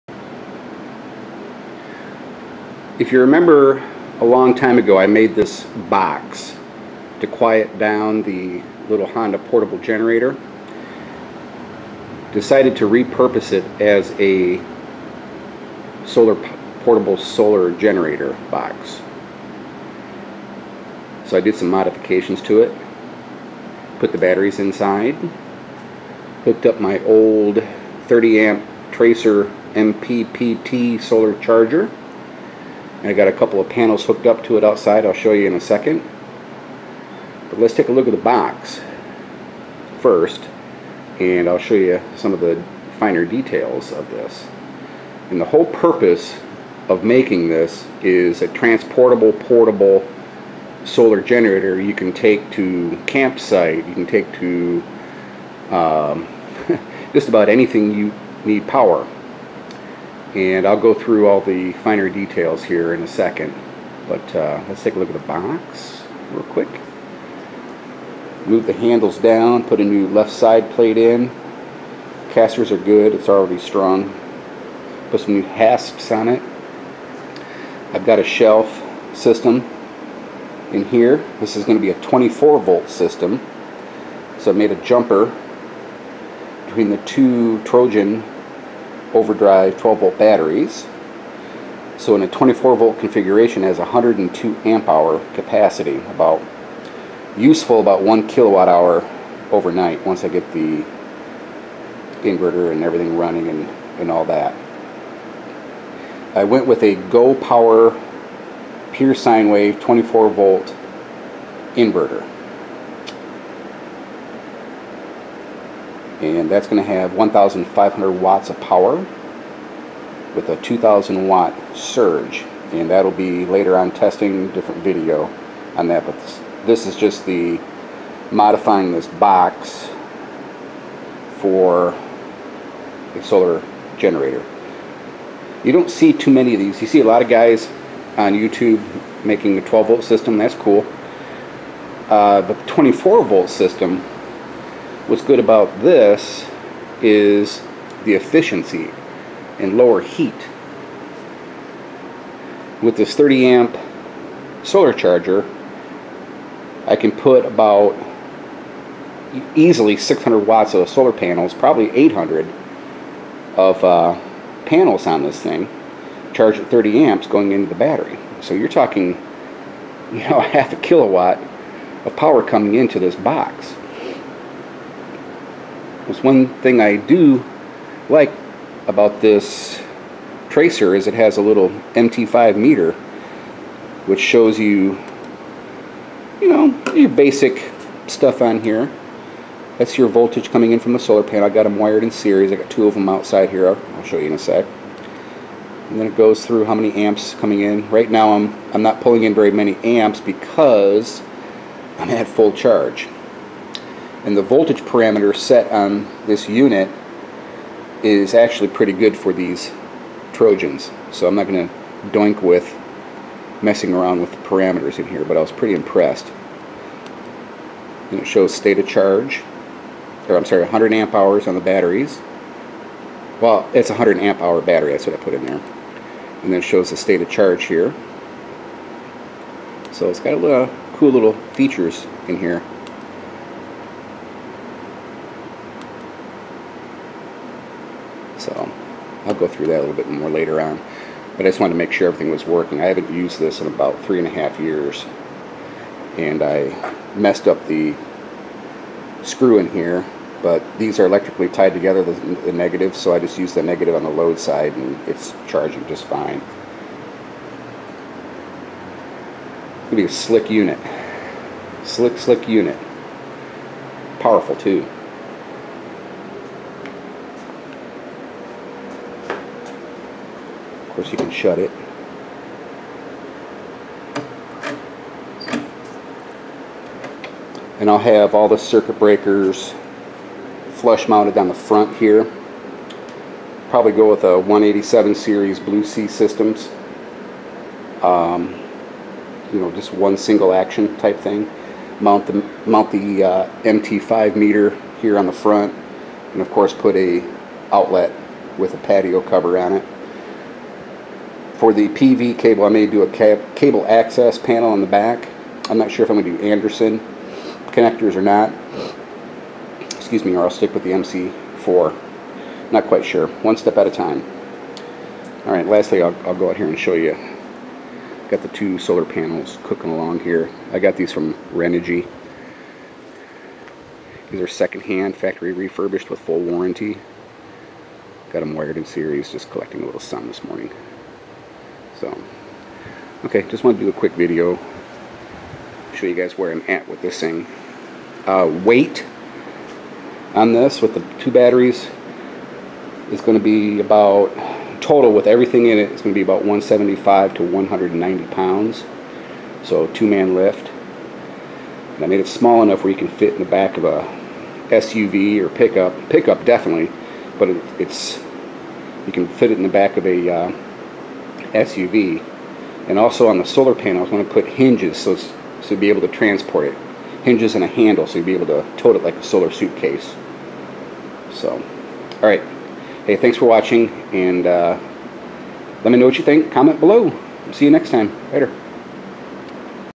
DIY portable solar generator (Part 2)
DIY 24v portable solar power generator. Got some work done on the box. The biggest obstacle was securing the batteries. I used a 400 lbs ratchet strap (very tight now...not going anywhere) and bolted it down. Then, made a shelf for the solar charger and inverter (AWP the inverter maybe next week). I installed a new left side panel (will have a flush mount dual AC fan system blowing air through the whole box). I installed new hasps and moved the lifting handles (added two) to the bottom end corners. The box will likely weigh in at about 175-190 lbs. Heavy, but it will be a 1,500 watt powerhouse. Fedex delivered two 100w solar panels...so, hooked them up and began testing the old EP Solar Tracer 30-amp MPPT solar charge controller. Why 24v? More efficient, less heat, and can feasibly handle 600-800 watts in solar panels. With max sun, six 100w solar panels could harvest 3+ KW/H's per day! There is also space for two more batteries. Gotta plan for expansion!

Blue Sea Systems 187-Series Circuit Breaker (Amazon)
Blue Sea Systems Maxibus Bussbar 6-post (Amazon)

This Channel is NOT a democracy (free speech)...it is my discretion if I want you/your comments here.